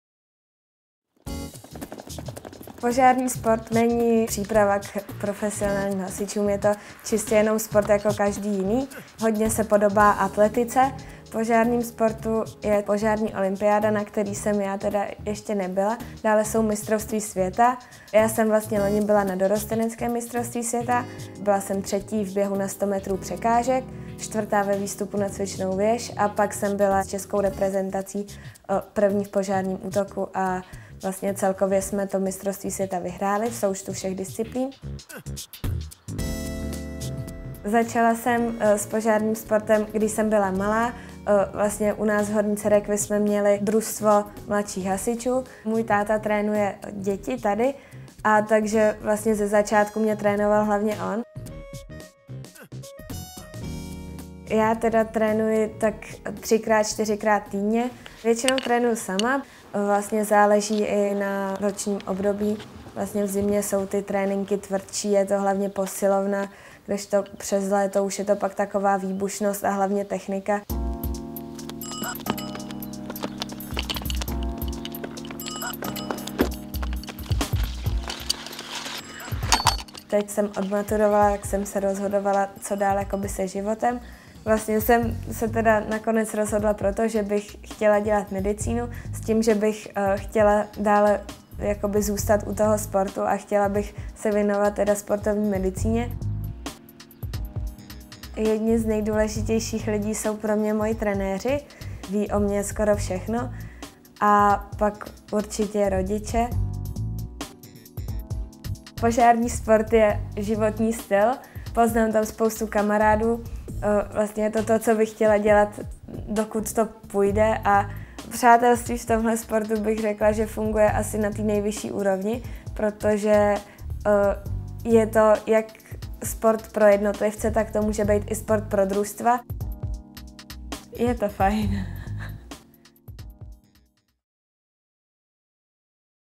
Nejvyšší ocenění Kraje Vysočina 2015: Kamila Bartošková
V roce 2015 získala dřevěnou medaili Kraje Vysočina.